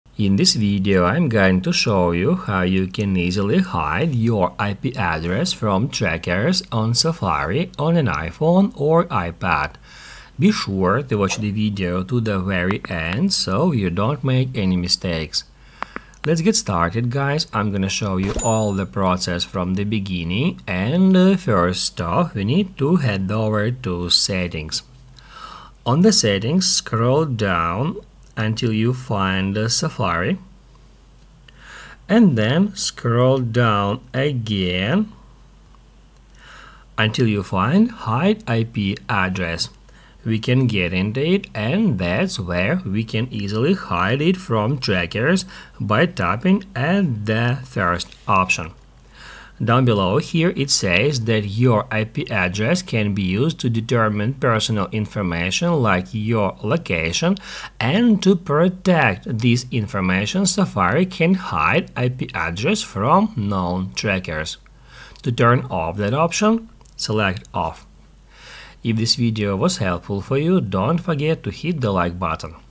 How to Turn On or Off Hide Ip Address From Trackers on Safari - [iPhone 15 Pro]
If you are looking for a video about how to turn on or off hide ip address from trackers on safari, here it is!